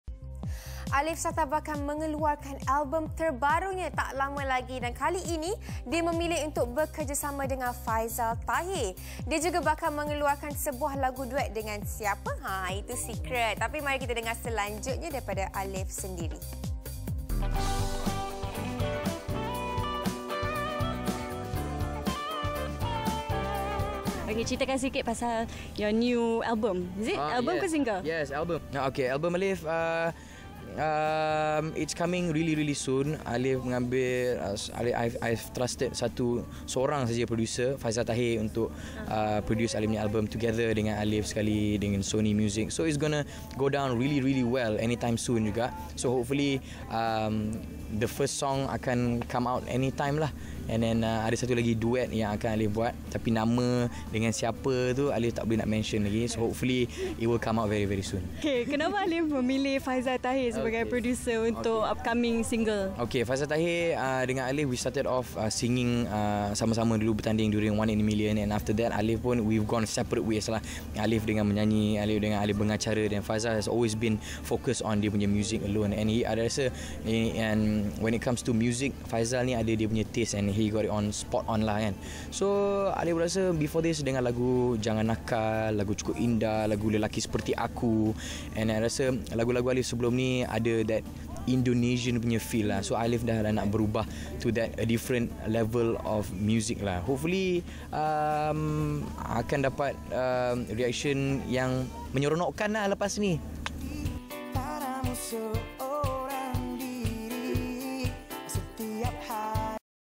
Alif Satar percaya Faizal Tahir
Alif Satar bakal mengeluarkan album baru tidak lama lagi. Dia memilih untuk bekerjasama denga Faizal Tahir.